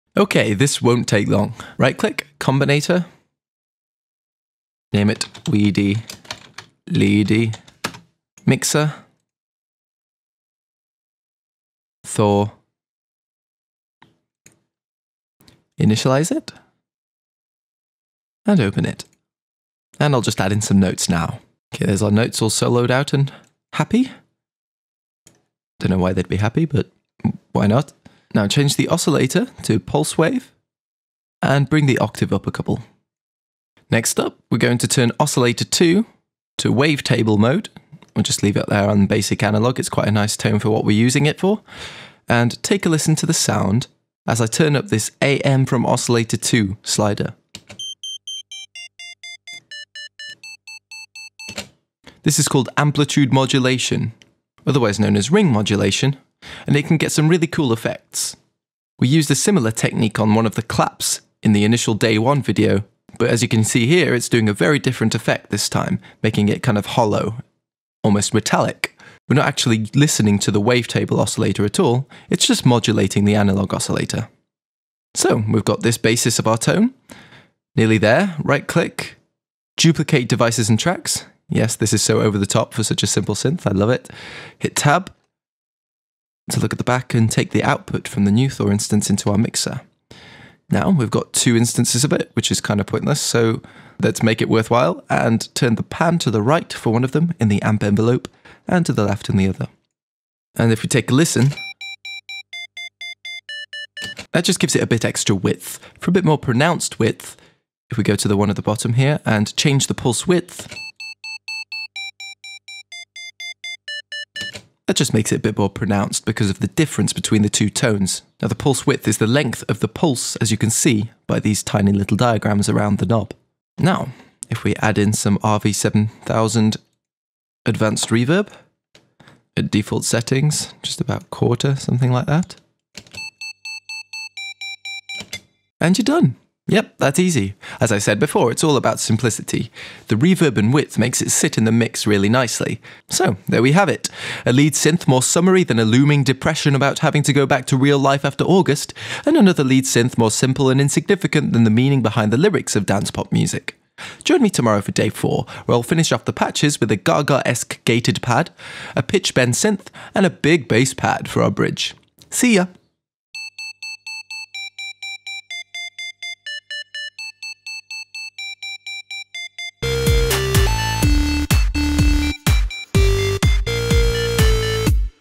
Dance Pop Tutorial Day 3 pt 2: blippy lead synth [7 Day Song]
for the forum (Shocking, right?)

- Check it out for more info on Propellerhead's music making month!

This Reason 5 Tutorial is for producers looking to get into Chart-tastic Dance Pop like Lady Gaga, Ke$ha, Katy Perry, Taio Cruise... basically anything produced by Max Martin or Dr Luke.   Incredible producers.

If you use fruity loops or garage band or logic, protools, ableton or any other DAW I'm sure you'll find something in here you can learn from, since I try to explain *why* I do things rather than just *what* I do.

Let me know what you think of this 7 day song!